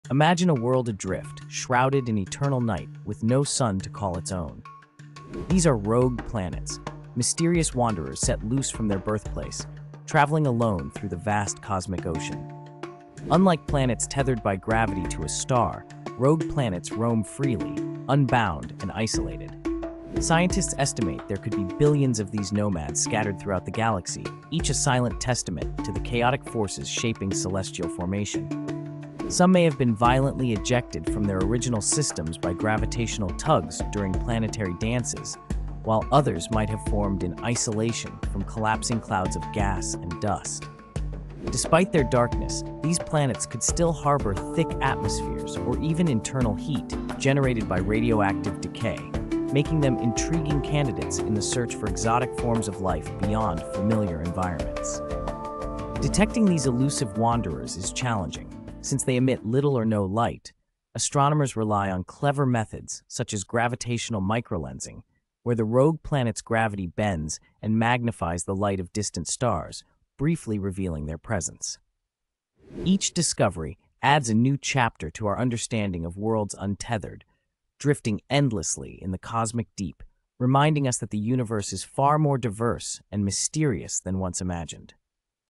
Rogue Planets: Cosmic Drifters Unveiled
Meet the mysterious rogue planets—cosmic wanderers cut loose from their homes. What secrets do these lonely worlds hold?